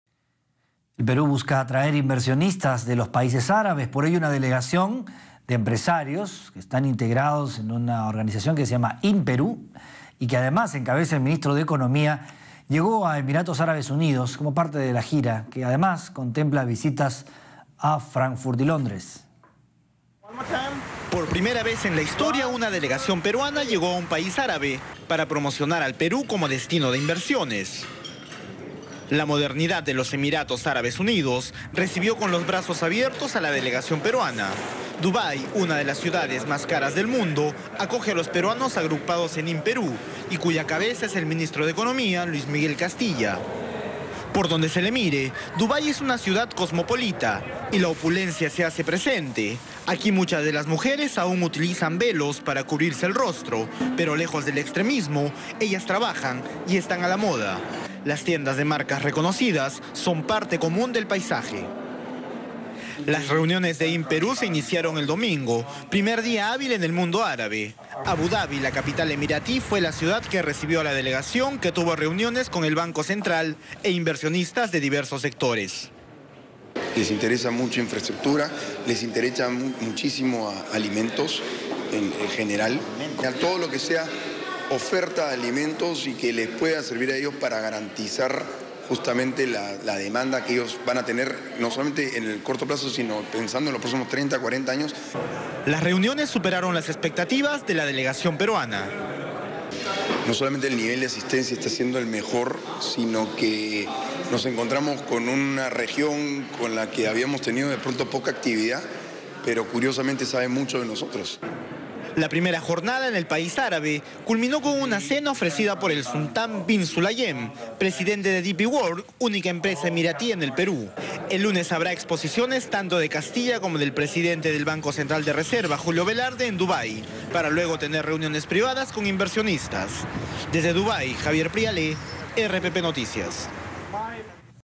25/07/2013 - RPP TV NOTICIAS - PERÚ EN BUSCA DE INVERSIONES ÁRABES
Delegación peruana "In Perú", encabezada por el ministro de Economía Luis Miguel Castilla, inició gira en Emiratos Árabes Unidos.